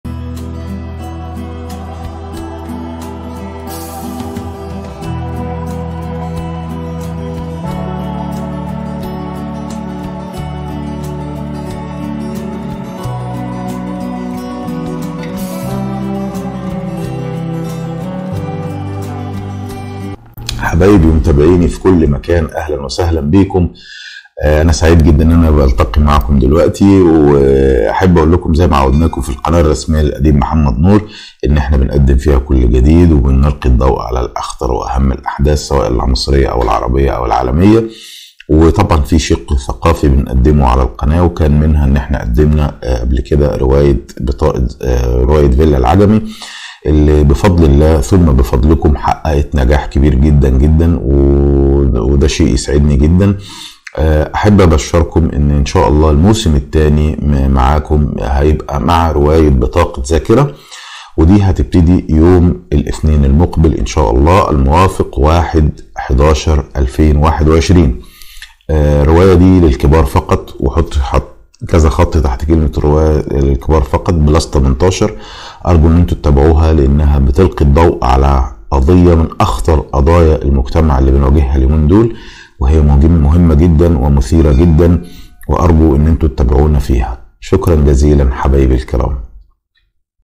برجاء الإنتباه 🔴 إعلان هام جداً جداً 🔴 للكبار فقط 🔴 +18
برجاء الانتباه.
إعلان هام جداً جداً..
يرجى مشاهدة هذا الفيديو للأهمية.
ولمشاهدة الموسم الثاني والذي ستعرض فيه رواية ( بطاقة ذاكرة )
وهي رواية للكبار فقط +18 .. ونكرر للكبار فقط..
 وهي الرواية التي أحدثت دوياً هائلاً سواءً على الصعيد المحلي أو العربي والتي سيتم بدء العرض لها بإذن الله تعالى من خلال برنامج..
  ( مع محمد نور )  بدءاً من يوم الأثنين الموافق
 1/ 11/ 2021